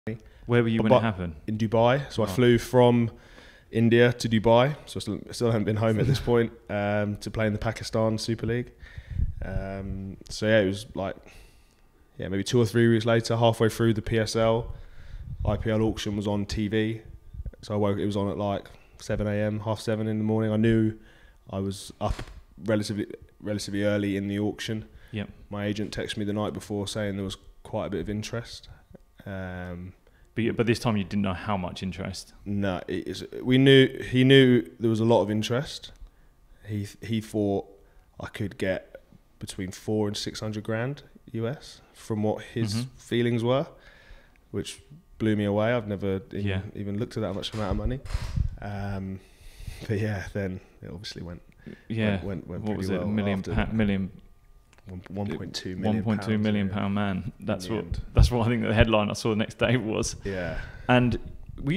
The Moment Tymal Mills Went For £1,000,000 In The IPL Auction
From E6 with Tymal Mills.

Download the MindStrong Sport App and start building a strong mind in sport now